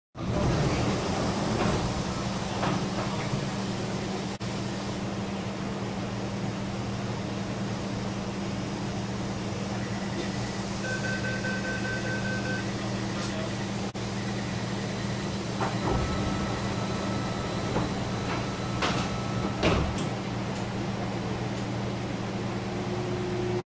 臺北捷運VAL256型膠輪電聯車車門開啟+關閉 | 2021/1/16 | Taipei MRT | 粘粘的鐵道攝影
拍攝時間:2021/1/16
拍攝地點:臺北捷運文湖線辛亥站
車型:VAL256型膠輪電聯車